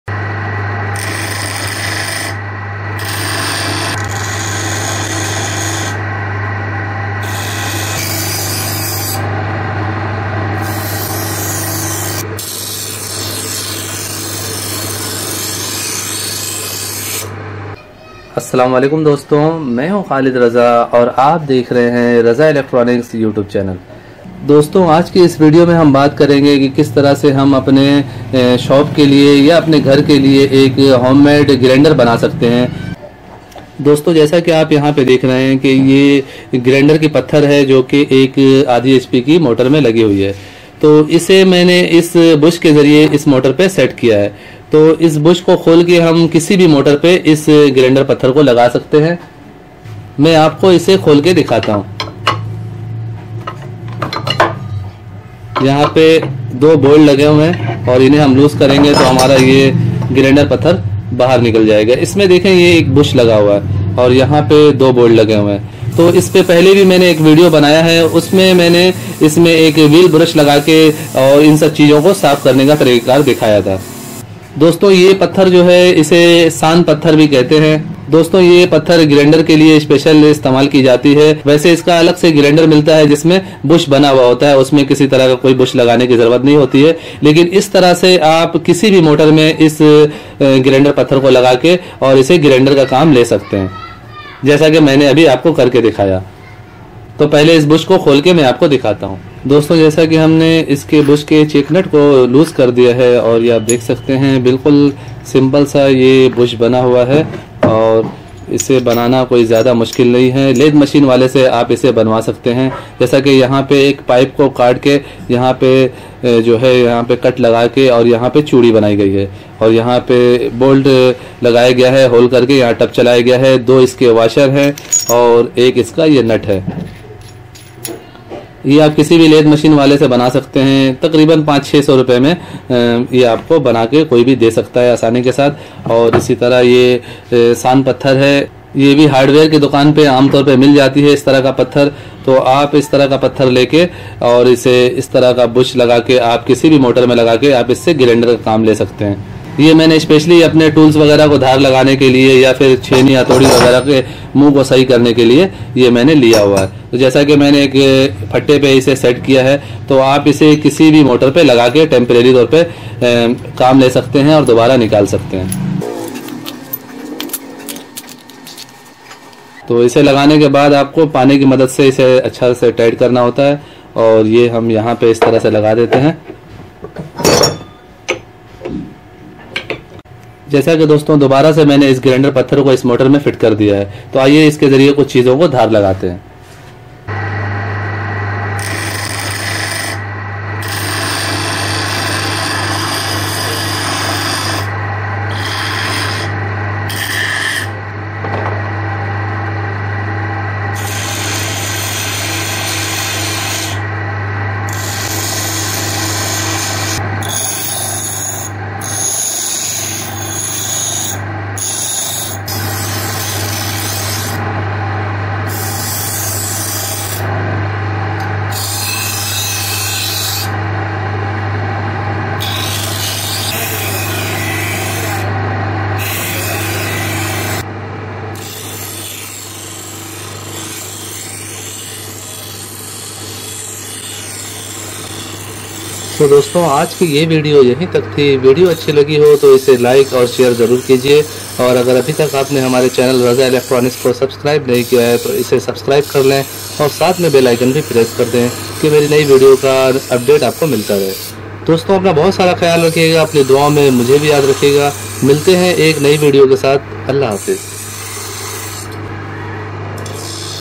How To Make Grinder At Home
is video me grinder ko kisi bhi motor me lagakr use karne ka tariqa bataya gaya he
i hope ke is video ko dekhne se ap ke knowledge me izafa hoga video achhi lage to isse like aur shear zaroor kare aur mere channel raza electronics ko subscribe karke bell button press karde